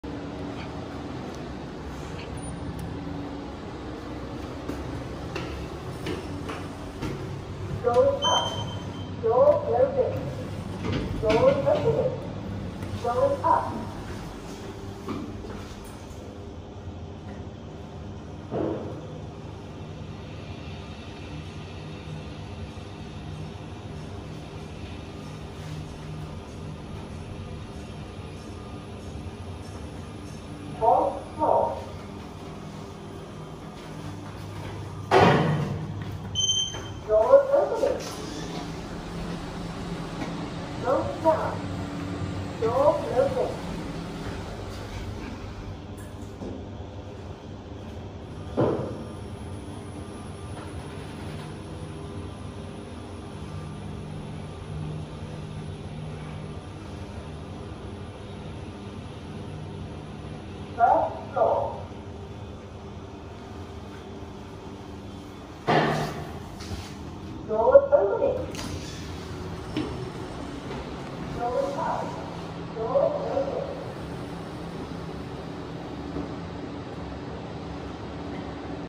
KONE S MONOSPACE MRL @ TEMASEK POLYTECHNIC (LIFT 51)
It's true that Temasek Polytechnic love Toshiba and Chevalier A LOT! Just look at the sheer number of Toshibas they have in their compound!

After the main compound was completed in the 90s, Temasek Polytechnic has infill developments. Since they REALLY love Chevalier, no surprise they got Toshiba back.

But Toshiba said no, we're out of stock. So they end up with a few Kones. Or that Temasek Poly selected a different bidder this time round.